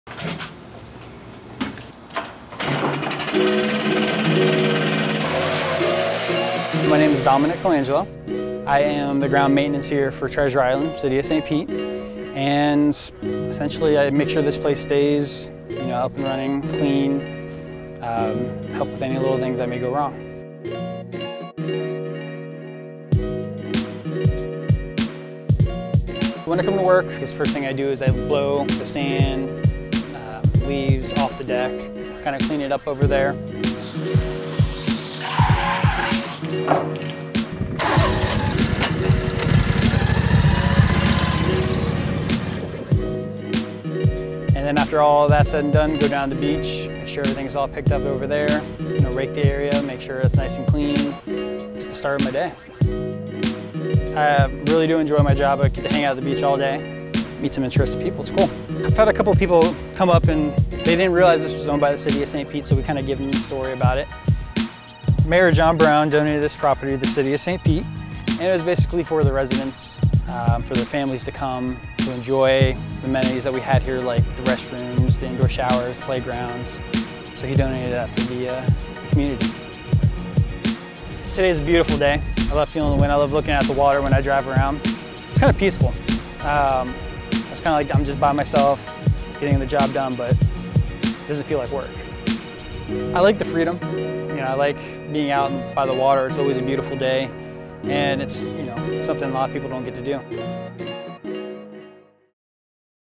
On The Job: Treasure Island Beach | St. Pete, FL
Surprisingly, the City of St. Pete owns a portion of Treasure Island Beach, which is available for public access with facilities and parking. Follow Dominic as he preps for the day and keeps the beach looking beautiful.